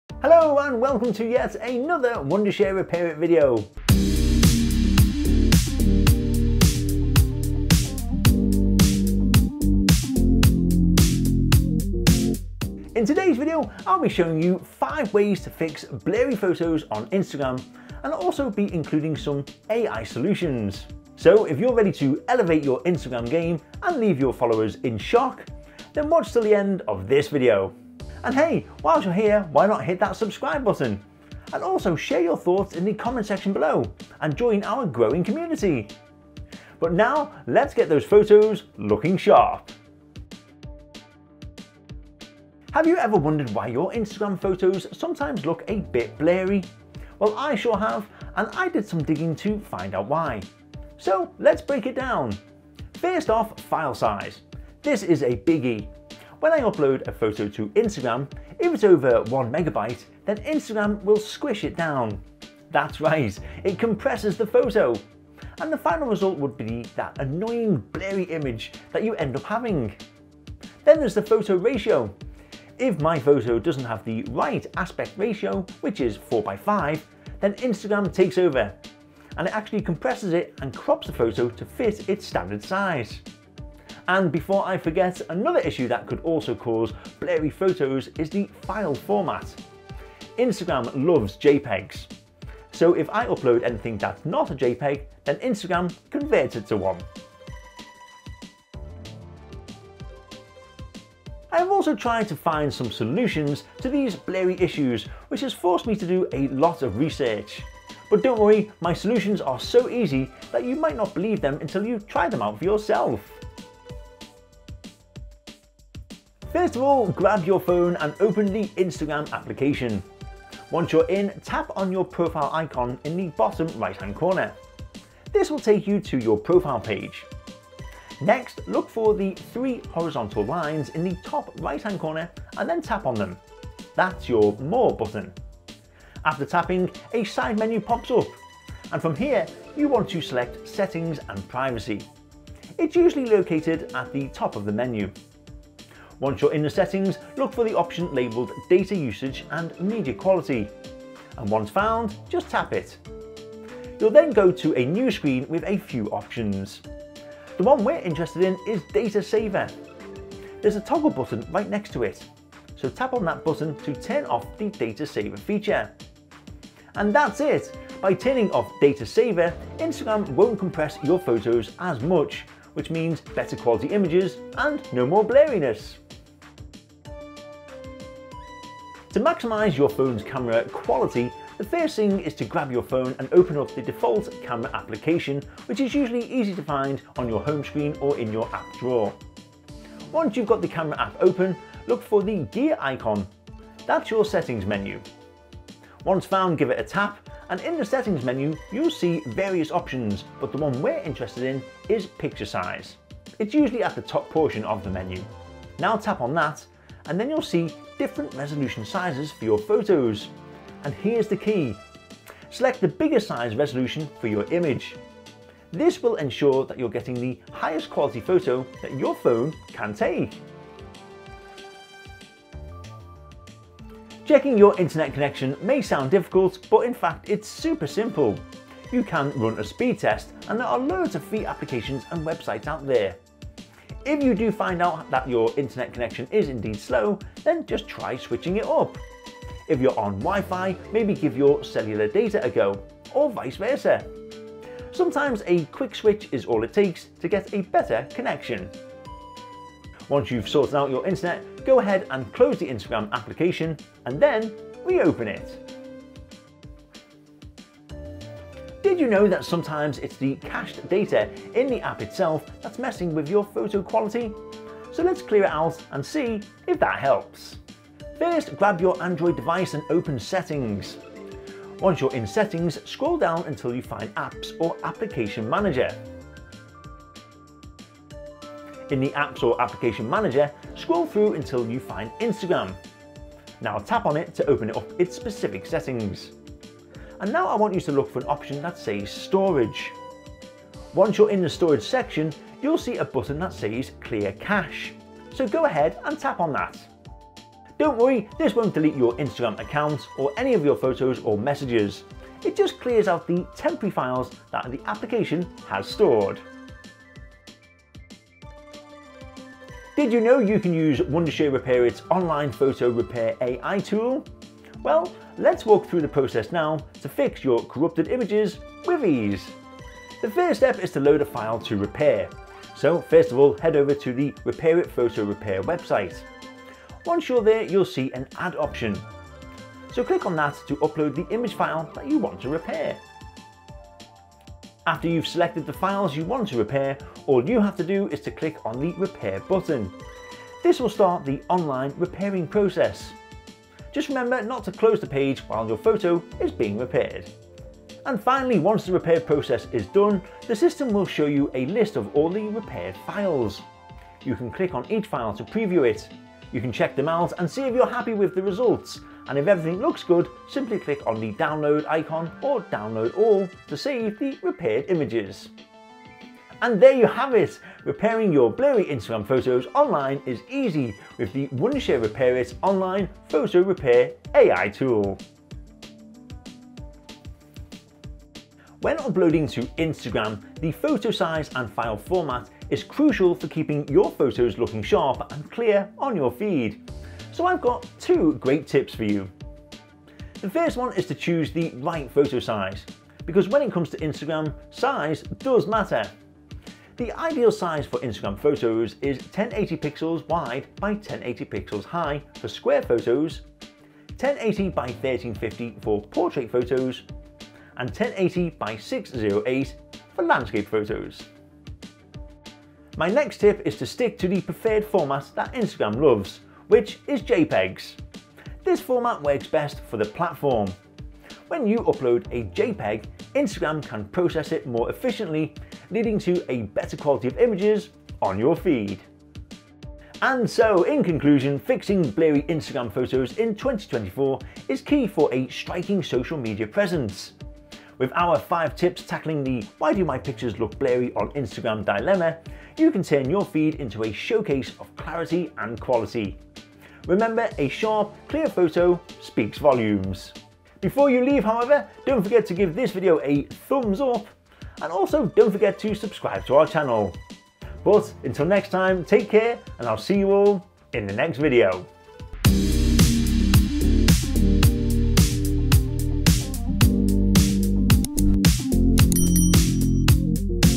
[2024 New]5 Ways to Fix Blurry Photos on Instagram — AI Solution Included
Ready to enhance your blurry Instagram photo? Learn How to fix blurry pictures on Instagram and make your visuals crystal clear.

Timestamps:
0:00 Intro
0:40 Why Do My Photos Look Blurry on Instagram?
1:55 Method 1: Instagram Data Setting
2:57 Method 2: Maximize Phones Camera Quality
3:42 Method 3: Check Your Internet Connection
4:17 Method 4: Clear Instagram Cache (Android)
5:08 Method 5: Wondershare Repairit Online Photo Repair AI Tool
6:16 Tips About Photo Size and Format When Uploading to Instagram

In today's digital world, having high-quality, visually appealing photos on your Instagram profile is a must. But sometimes, the platform's compression algorithm can cause your pictures to look blurry and less than perfect. That's where our tips come in! 🌟

From adjusting your Instagram settings to using photo editing apps, we've got plenty of solutions for you to try. Plus, we're introducing a cutting-edge AI-powered tool that'll help you take your Instagram game to the next level! 🤖🎨

No more worrying about blurry or pixelated images; follow our step-by-step guide and watch your photos transform into stunning, high-resolution masterpieces. 👍🔔